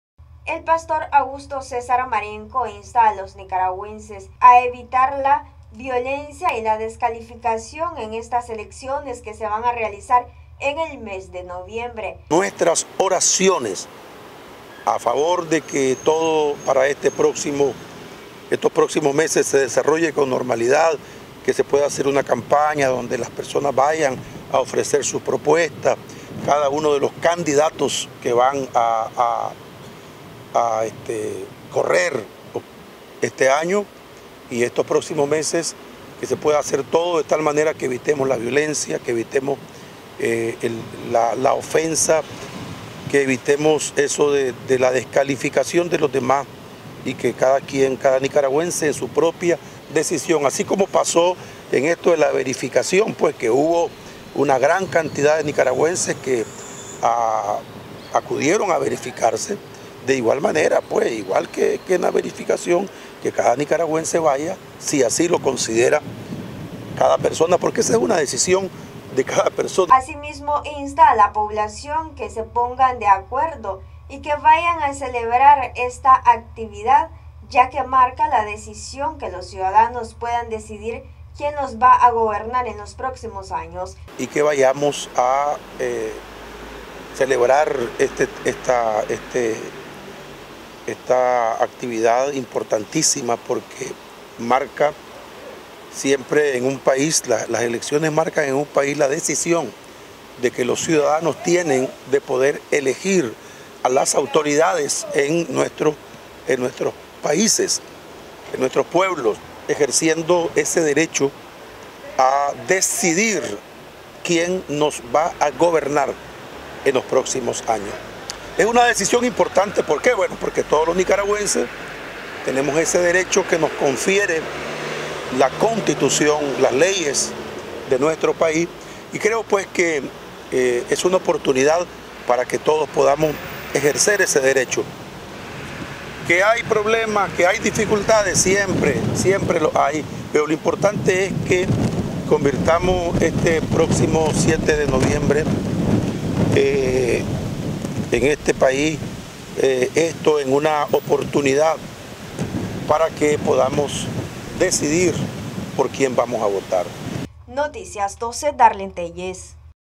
Pastor evangélico insta a los nicaragüenses a evitar la violencia en estas elecciones
Noticias12Nicaragua Pastor Evangélico Augusto César Marenco dijo que la función de la iglesia en estos momentos es orar por la nación y por las elecciones que se llevarán a cabo en noviembre, insta a elegir en el marco del respeto.